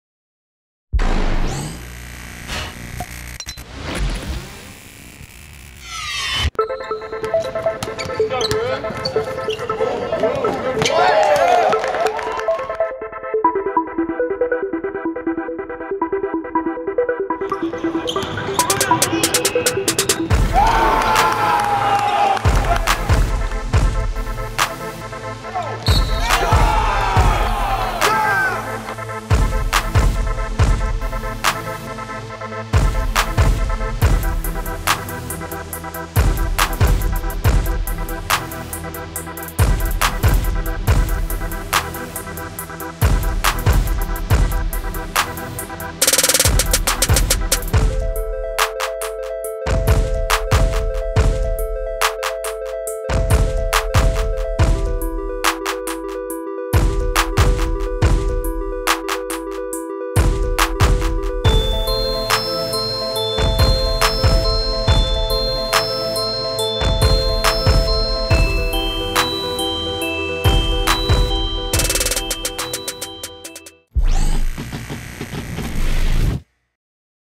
Adjehi Baru Highlights - North Carolina Target - Team Loaded - Class of 2011 - Steward School
Baru is a 6'9 Center from the Class of 2011.  He is being recruited by Maryland, North Carolina, USC, Virginia Tech, and College of Charleston.  He plays his AAU ball for Team Loaded and he attends the Steward School in Richmond Virginia.  Song by DDavisinthe6